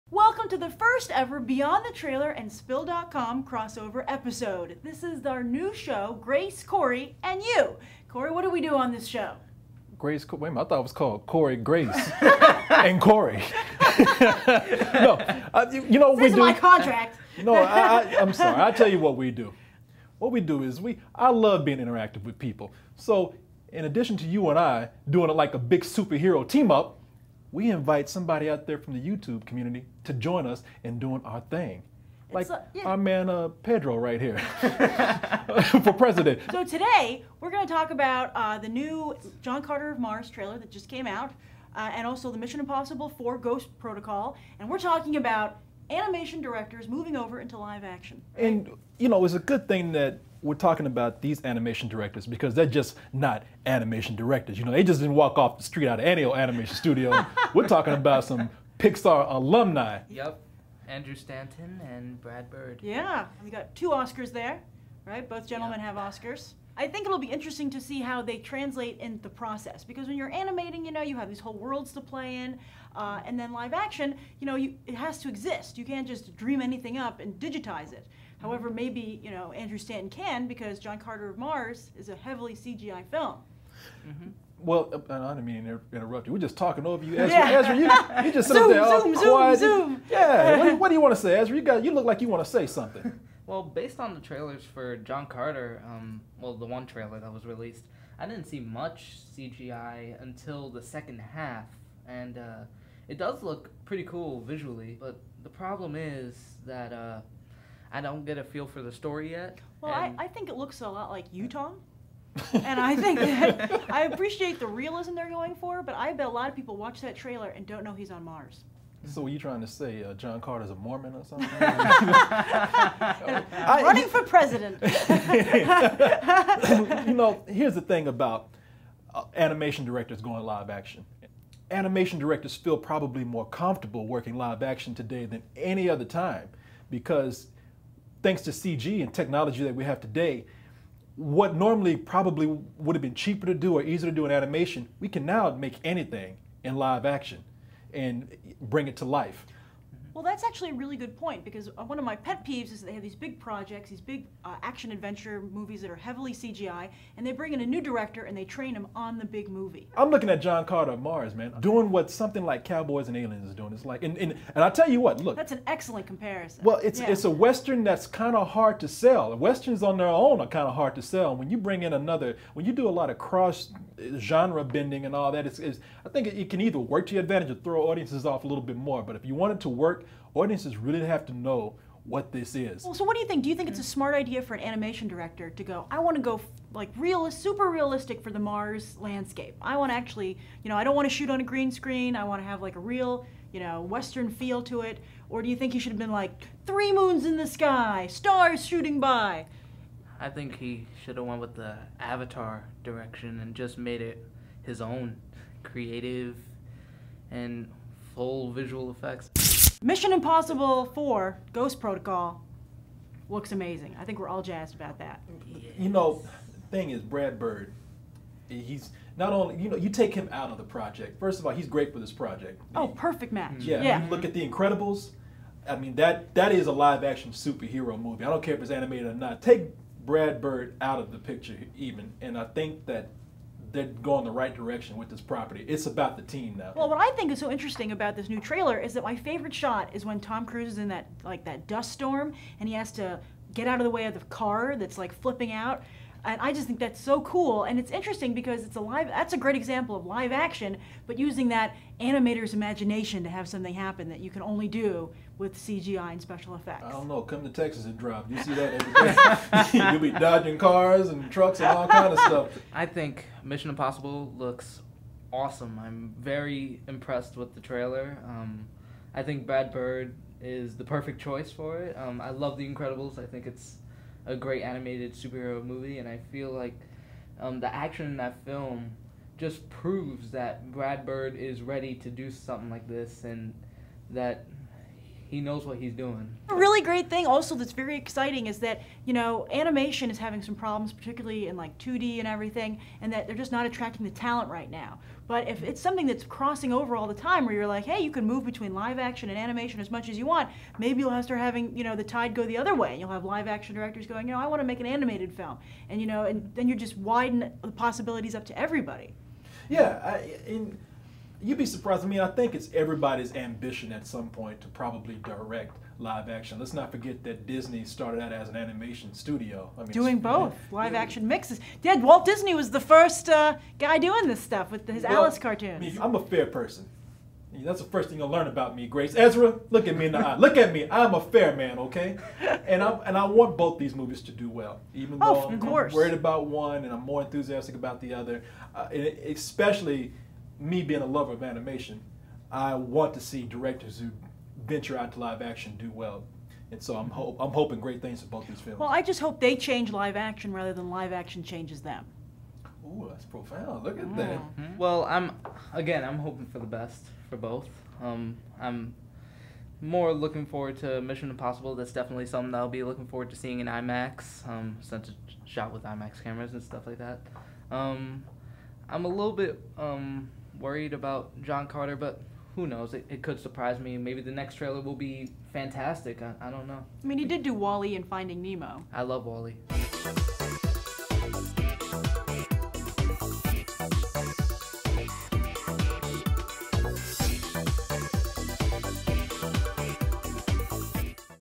Mission Impossible 4 and Disney's John Carter of Mars : Beyond The Trailer & Spill.com
Mission Impossible 4 Ghost Protocol and Disney's John Carter of Mars share one thing in common, and that's a Pixar animation director making his first live action movie!  Join Grace from Beyond The Trailer and Korey from Spill.com for their first ever crossover episode as they discuss Brad Bird's Mission Impossible Ghost Protocol and Andrew Stanton's John Carter of Mars!  Enjoy Mission Impossible 4 Ghost Protocol and Disney's John Carter of Mars!